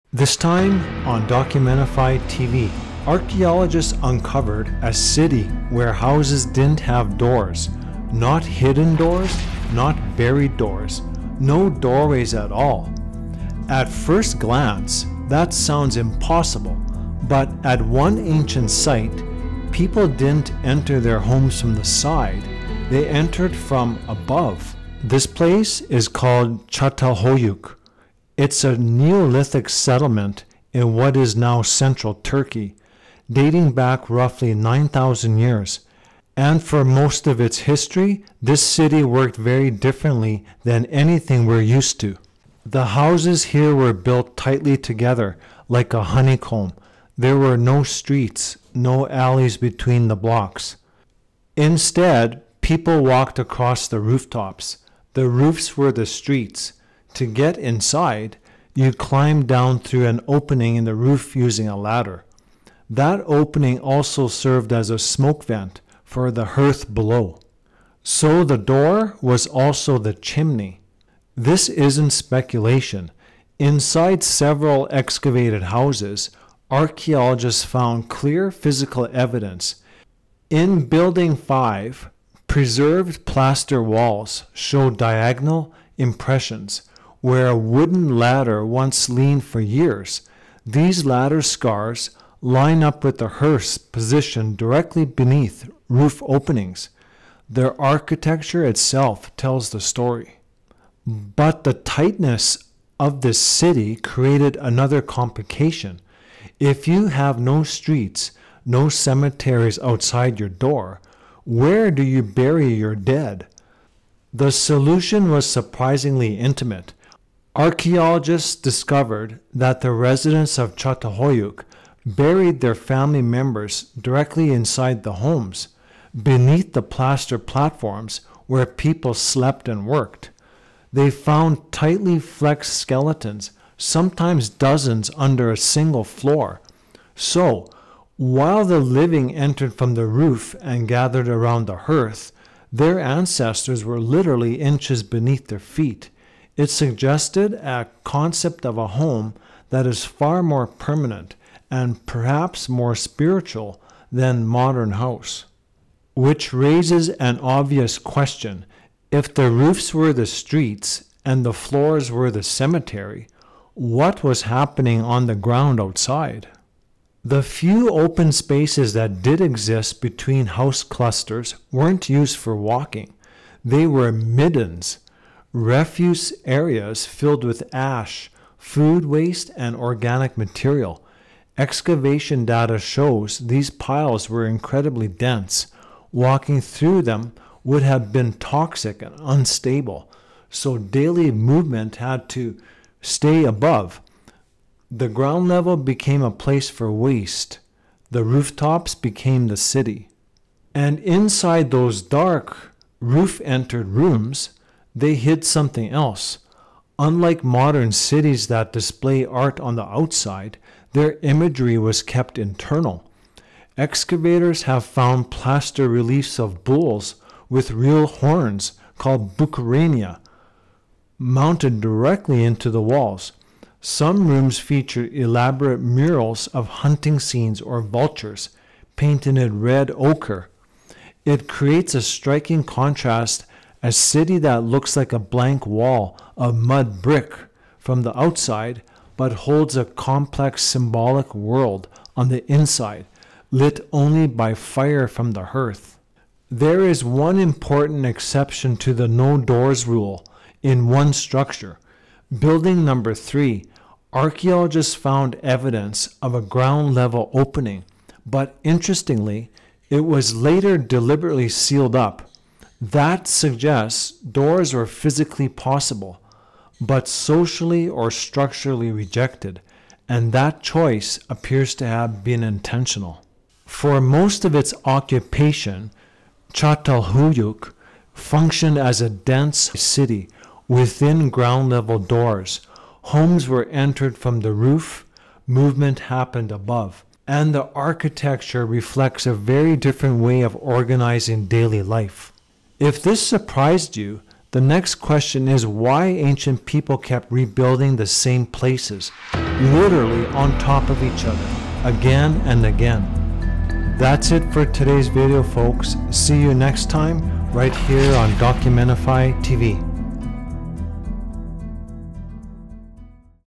9,000-Year-Old City Found Buried — Why Are There No Doors?
A Neolithic city with no streets and no doors shouldn’t work — but it did.

At the 9,000-year-old site of Çatalhöyük in modern Turkey, archaeologists uncovered a settlement designed around rooftop movement and roof-entry homes.

Archaeologists excavating the Neolithic site of Çatalhöyük uncovered a structural anomaly that defies modern city planning. The houses were built wall-to-wall like a honeycomb, with no alleys or walking paths between them. Instead, evidence confirms that residents treated the rooftops as streets and entered their homes through smoke holes using ladders.

But this “roof life” created deeper consequences. Inside these sealed homes, excavations revealed ancestors buried directly beneath the sleeping platforms, and symbolic art kept entirely inside the domestic space. This video examines the forensic evidence behind the city without doorways — including preserved ladder scars, midden zones that replaced streets, and the deliberate sealing of a ground-level opening in Building 3.

Key Topics Covered:
 • Roof-entry architecture
 • Ladder scar evidence (Building 5)
 • Why middens replaced streets
 • Burials beneath domestic floors
 • The sealed doorway anomaly (Building 3)
Arc: Old World / Lost Technology
 System: Documentify TV

PHOTO CREDITS & ATTRIBUTION

Images & Footage: • Primary Ladder Evidence: Arkeolojik Haber (Neolithic Wooden Ladder Discovery)

Research Notes: • The Official Excavation: Çatalhöyük Research Project (catalhoyuk.com) • UNESCO World Heritage Listing: "Neolithic Site of Çatalhöyük" • The "History of Houses": Hodder, I. (2006). The Leopard's Tale: Revealing the Mysteries of Çatalhöyük. Thames & Hudson.